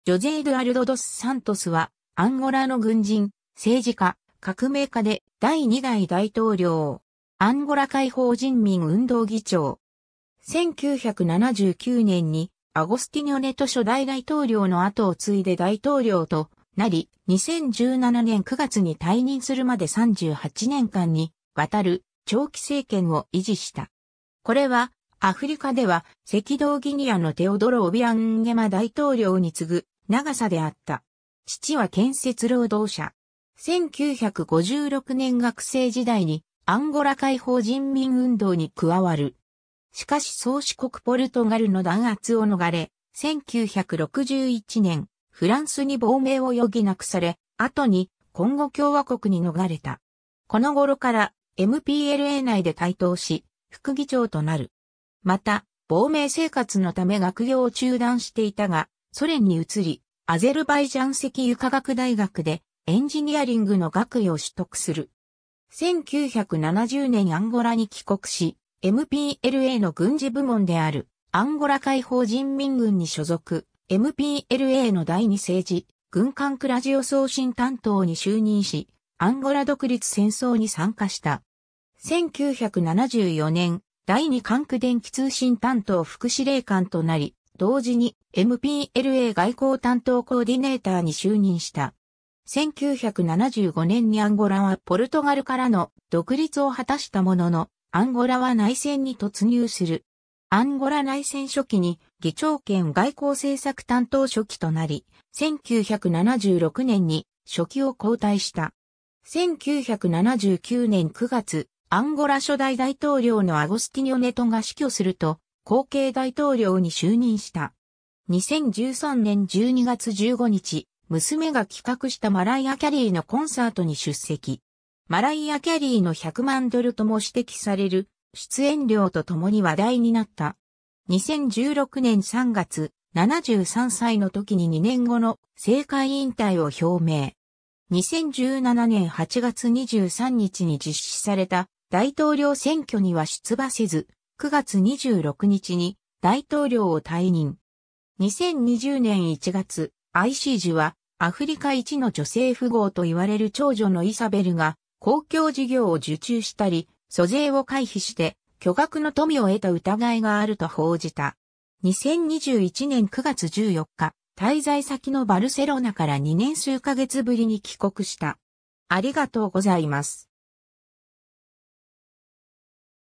ジョゼ・エドゥアルド・ドス・サントス
ジョゼ・エドゥアルド・ドス・サントス（ポルトガル語: José Eduardo dos Santos, 1942年8月28日 - ）は、アンゴラの軍人、政治家、革命家で第2代大統領。
アンゴラ解放人民運動(MPLA)議長。
1979年にアゴスティニョ・ネト初代大統領の後を継いで大統領となり、2017年9月に退任するまで38年間にわたる長期政権を維持した。
これはアフリカでは赤道ギニアのテオドロ・オビアン・ンゲマ大統領に次ぐ長さであった。
 父は建設労働者。
1956年学生時代にアンゴラ解放人民運動（MPLA）に加わる。
しかし宗主国ポルトガルの弾圧を逃れ、1961年フランスに亡命を余儀なくされ、後にコンゴ共和国に逃れた。
この頃からMPLA内で台頭し、副議長となる。
また、亡命生活のため学業を中断していたが、ソ連に移りアゼルバイジャン石油化学大学（現・アゼルバイジャン国立石油アカデミー、Azerbaijan State Oil Academy）でエンジニアリングの学位を取得する。
1970年アンゴラに帰国し、MPLAの軍事部門であるアンゴラ解放人民軍（Exército Popular de Libertação de Angola、EPLA）に所属、MPLAの第二政治・軍管区ラジオ送信担当に就任し、アンゴラ独立戦争に参加した。
1974年第二管区電気通信担当副司令官となり、同時にMPLA外交担当コーディネーターに就任した。
 1975年にアンゴラはポルトガルからの独立を果たしたものの、アンゴラは内戦に突入する。
アンゴラ内戦初期に議長兼外交政策担当書記となり、1976年に書記を交代した。
 1979年9月、アンゴラ初代大統領のアゴスティニョ・ネトが死去すると、後継大統領に就任した。
 2013年12月15日、娘が企画したマライア・キャリーのコンサートに出席。
マライア・キャリーの百万ドル（約1億円）とも指摘される出演料とともに話題になった。
 2016年3月、73歳の時に2年後の政界引退を表明。
2017年8月23日に実施された大統領選挙には出馬せず、9月26日に大統領を退任。
 2020年1月、ICIJはアフリカ一の女性富豪（資産約2300億円）と言われる長女のイサベルが公共事業を受注したり、租税を回避して巨額の富を得た疑いがあると報じた。
 2021年9月14日、滞在先のバルセロナから2年数か月ぶりに帰国した。